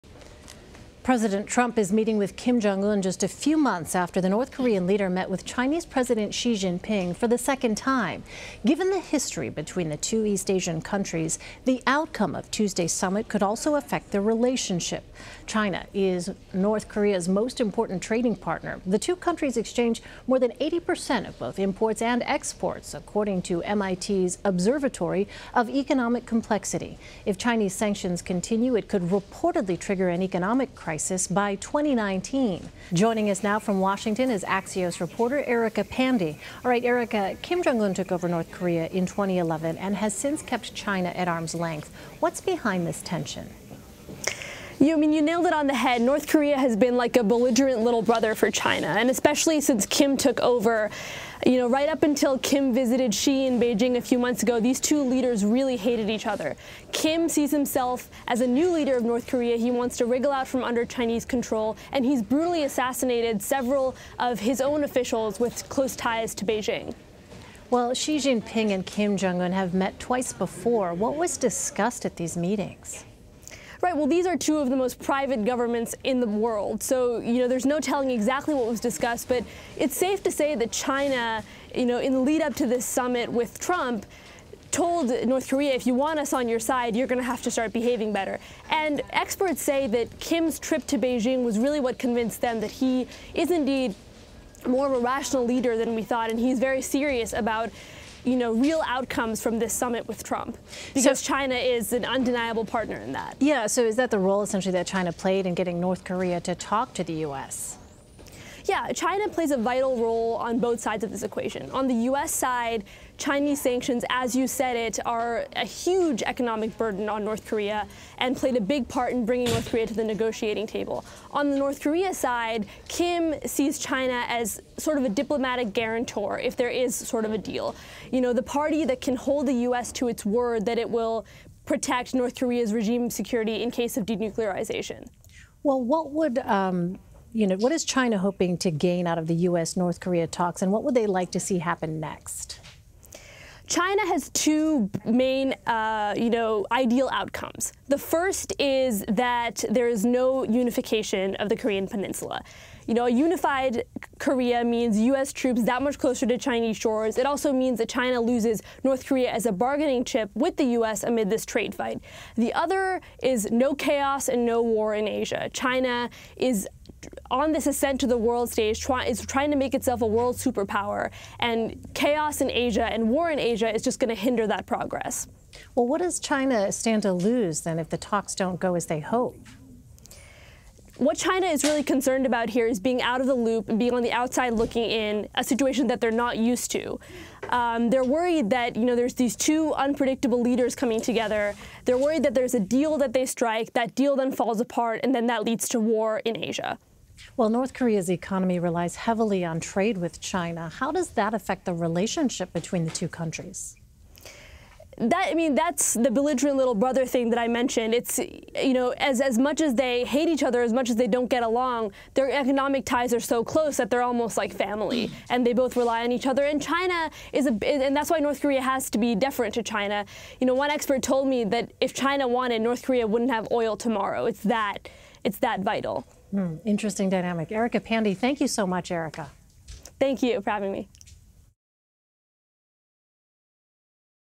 North Korea, China have complex relationship that can be affected by summit
China is North Korea's most important trading partner, accounting for more than 80 percent of both imports and exports for the regime. If sanctions continue, analysts say they could trigger an economic crisis for North Korea by next year. Axios reporter Erica Pandey joins CBSN with more.